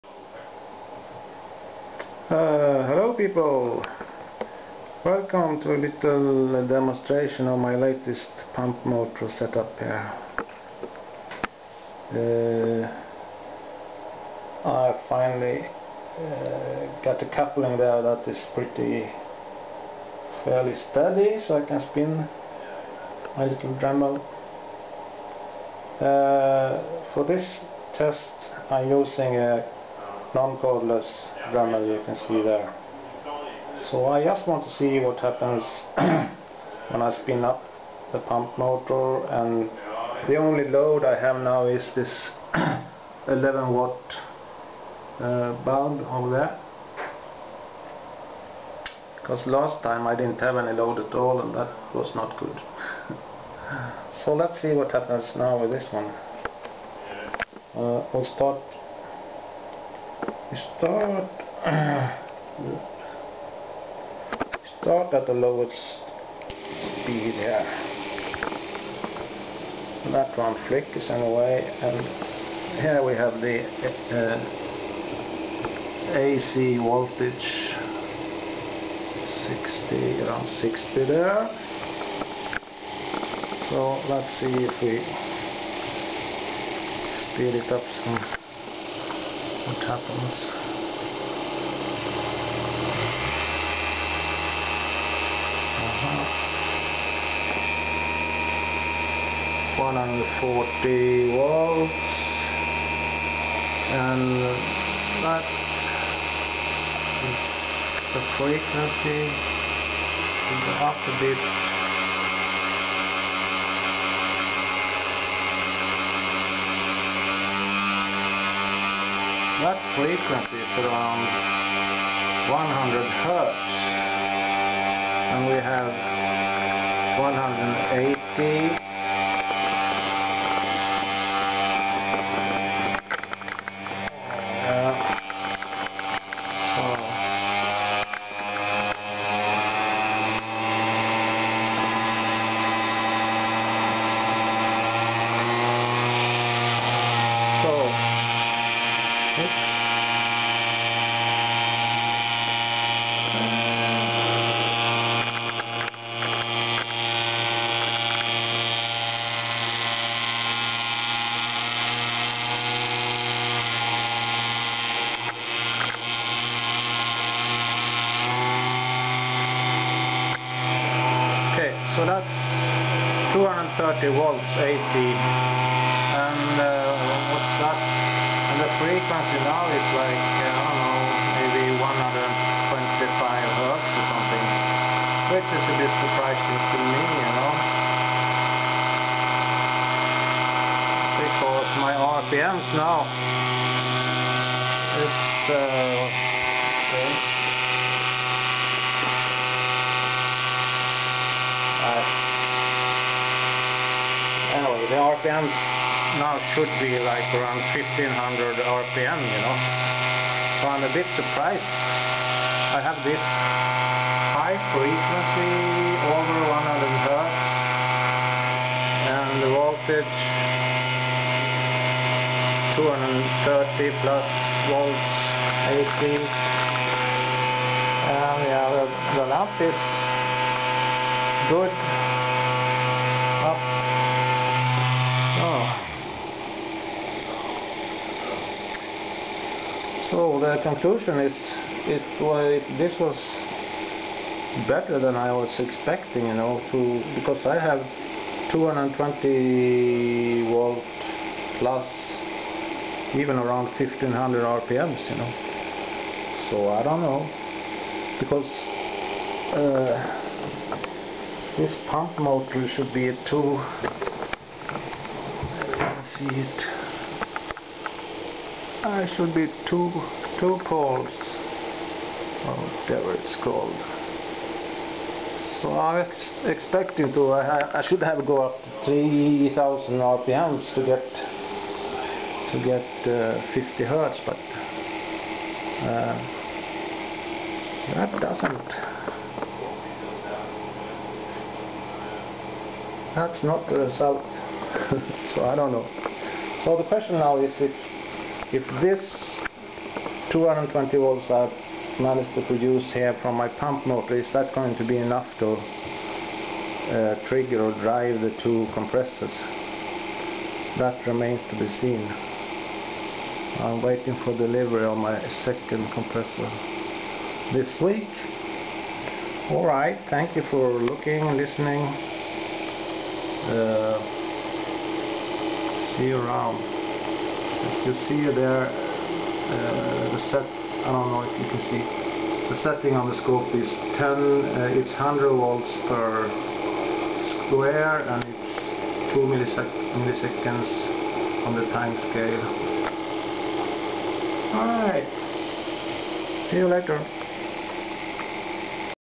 Magrav pump motor II
Pump motor is 220V / 50hz  , 23Watt
Dremel is model 4000   (max 4000rpm)

Added finding: I changed my 11W lamp to an old type 30 W lamp, pure resistive. Then I could only get up to 170 V AC daring some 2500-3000 rpm. This time I had a perfect sinus wave on the scope .
So guessing this won't be able to give enough power to the 2 compressors to get started